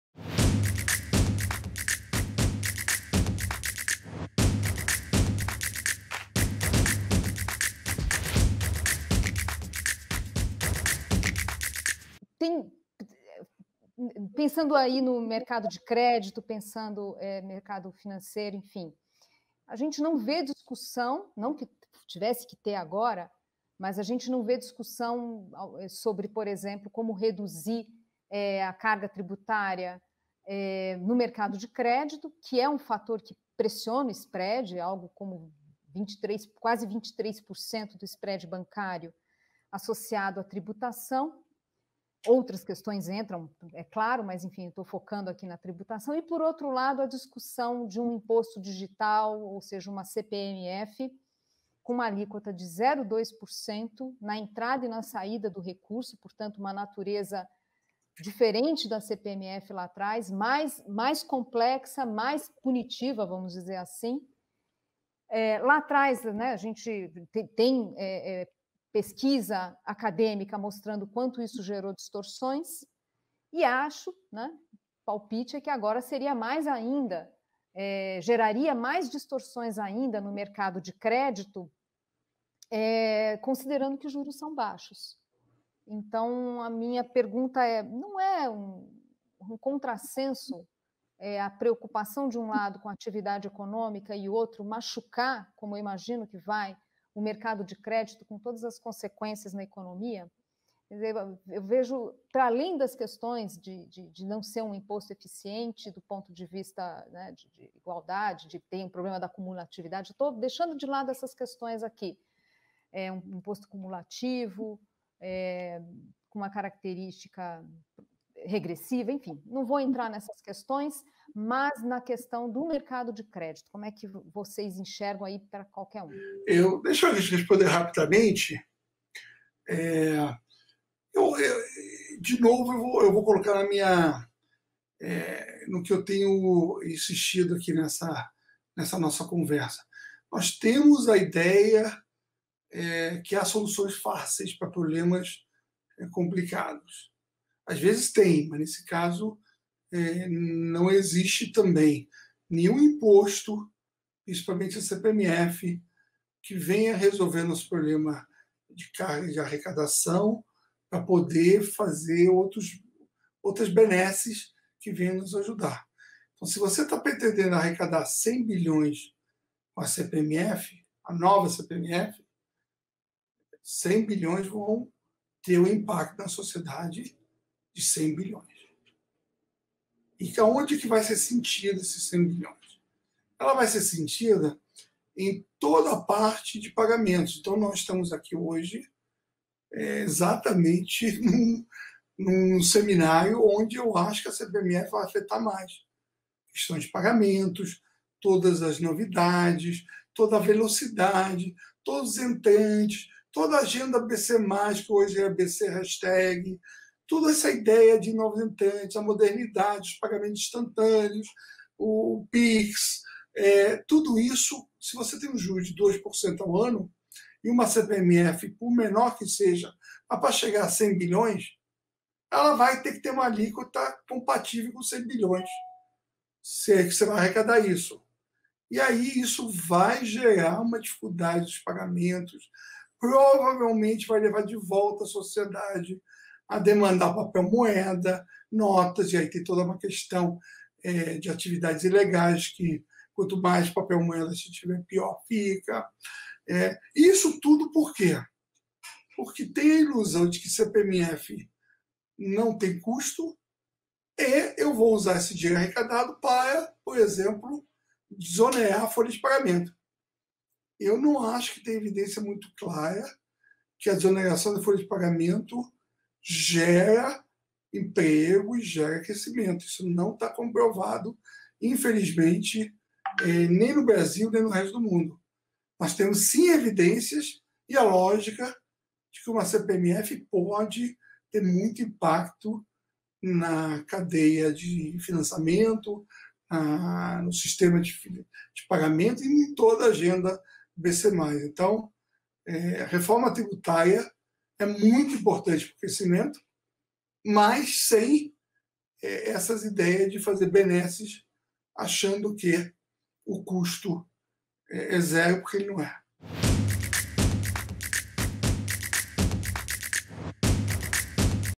Ilan Goldfajn: o impacto da nova CPMF no mercado e na economia
Com o apoio da @stone e da @exame, realizamos o nosso evento anual 100% online e gratuito: Retomada Econômica, Tecnologia e Inclusão Financeira.

Convidamos os principais nomes do país para debater a crise atual e seus reflexos no conjunto de instituições financeiras, cidadãos e empresas no Brasil.

Convidados:
Ilan Goldfjan: Ex-Presidente do Banco Central e Presidente do Conselho do Credit Suisse.
Zeina Latif: Consultora na Gibraltar Consultoria e Ex-economista-chefe da XP Investimentos.

00:00:00 Zeina Latif
00:02:06 Ilan Goldfjan